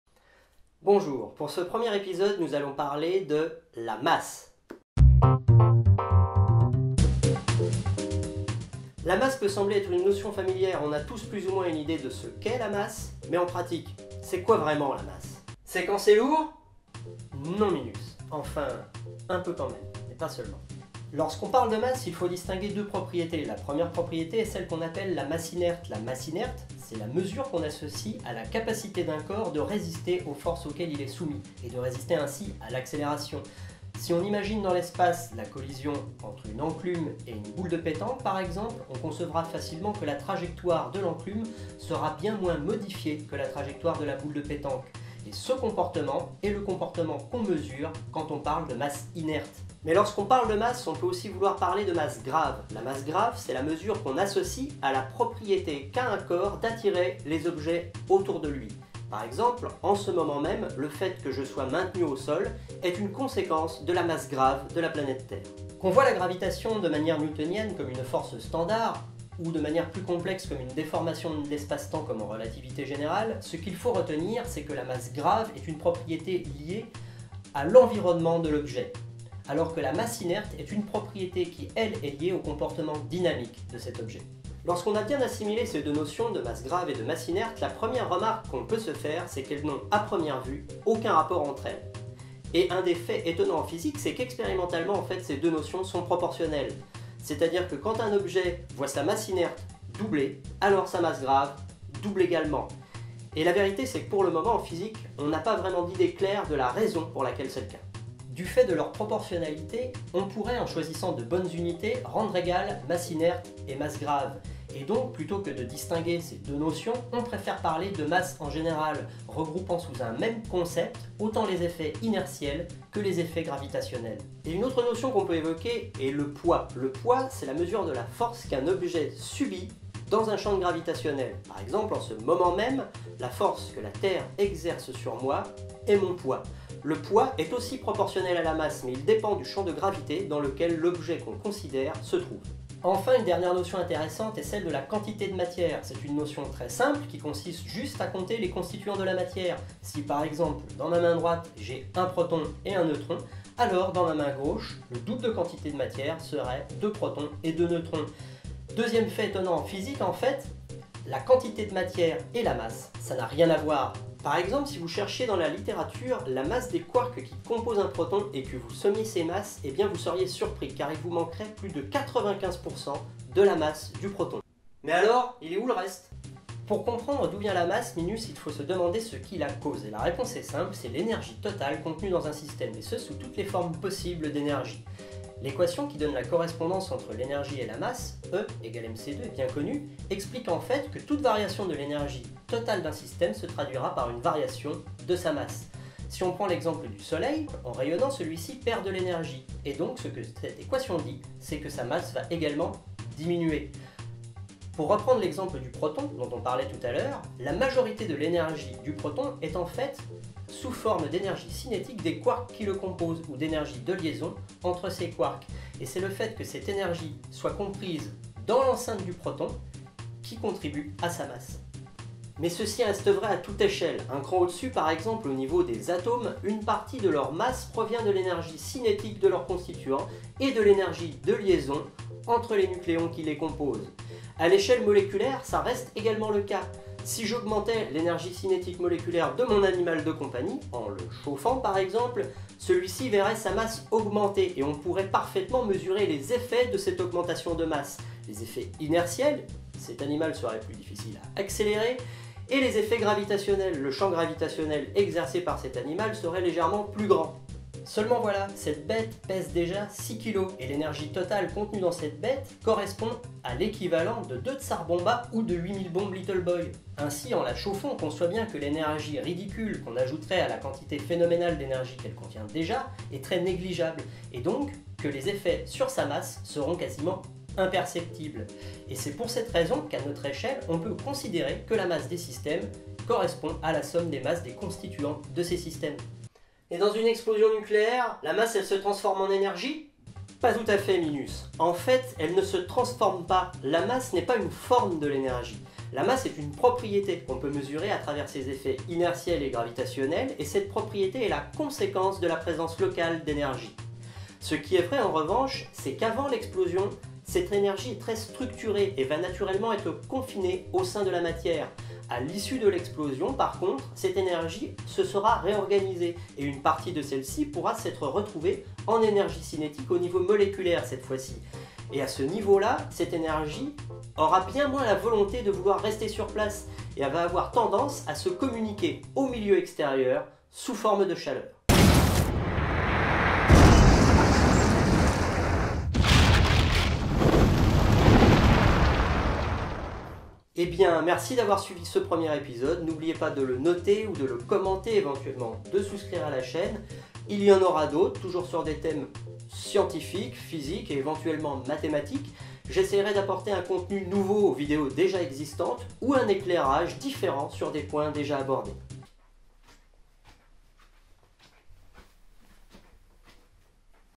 Qu'est ce que la Masse en physique fondamentale ? - Passe-science #1
Vous croyez savoir ce qu'est la masse? Si on vous posait la question qu'en diriez vous? Dans cette vidéo nous verrons en quelques minutes les concepts les plus importants autour de la notion de masse et son lien avec l’énergie.

Pour en savoir plus:

Vous pouvez me poser des questions en commentaires, tant que ça reste gérable j'y répondrais avec plaisir.